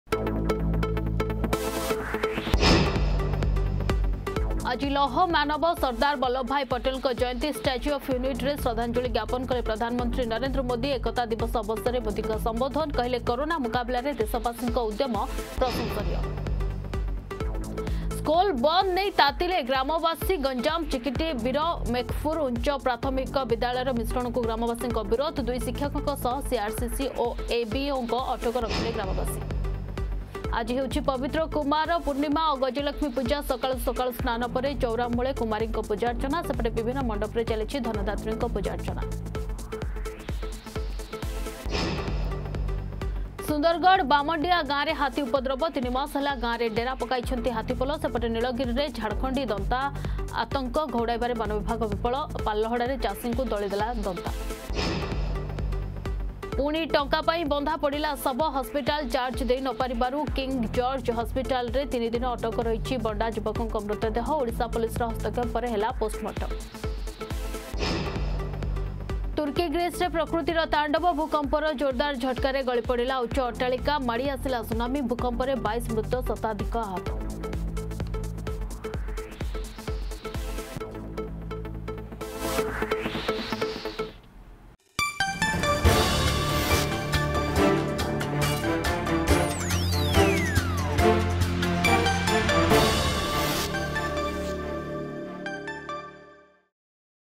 Headlines 10Am
ଷ୍ଟାଚ୍ୟୁ ଅଫ୍ ୟୁନିଟିରେ ଶ୍ରଦ୍ଧାଞ୍ଜଳି ଜ୍ଞାପନ କଲେ ପ୍ରଧାନମନ୍ତ୍ରୀ ନରେନ୍ଦ୍ର ମୋଦି. ସ୍କୁଲ ବନ୍ଦ ନେଇ ତାତିଲେ ଗ୍ରାମବାସୀ... ପୁଣି ଟଙ୍କା ପାଇଁ ବନ୍ଧା ପଡିଲା ଶବ... ତୁର୍କୀ-ଗ୍ରୀସରେ ପ୍ରକୃତିର ତାଣ୍ଡବ.